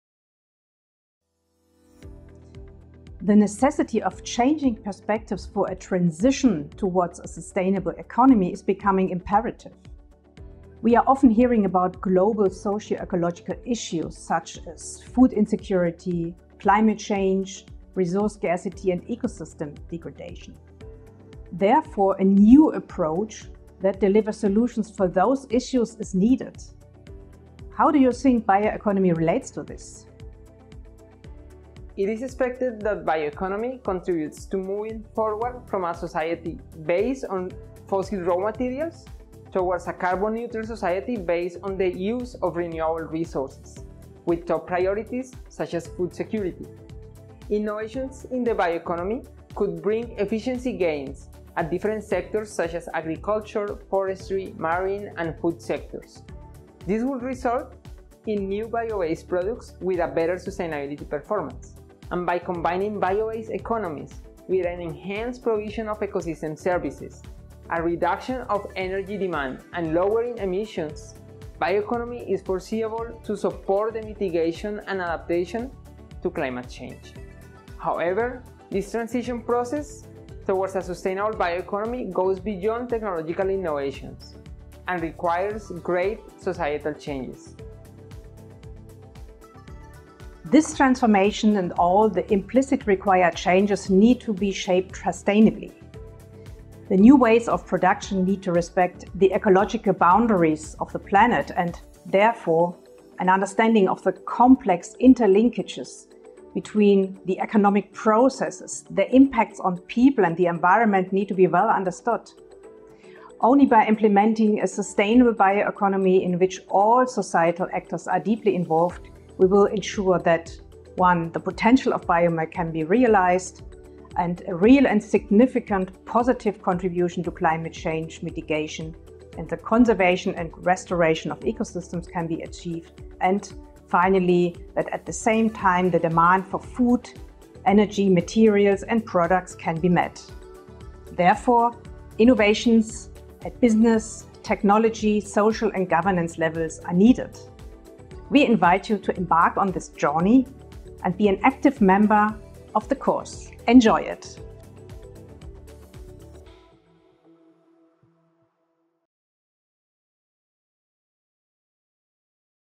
New online course: Concepts of Sustainable Bioeconomy by the University of Hohenheim
The open online course "Concepts of Sustainable Bioeconomy", developed by the University of Hohenheim, describes the bioeconomy and its underlying concepts from a meta-level. It addresses key questions such as: How can bioeconomy contribute to sustainability? What is the role of innovation and knowledge in the bioeconomy? What is necessary for the transition towards a sustainable bioeconomy?

This course was developed as part of the strategic partnership ABBEE (Accelerating the transition towards the Bio-Based Economy via Education), funded by the Erasmus+ program. It is one of four courses offered by leading European universities in the field of Bioeconomy

Video/Footage: ABBEE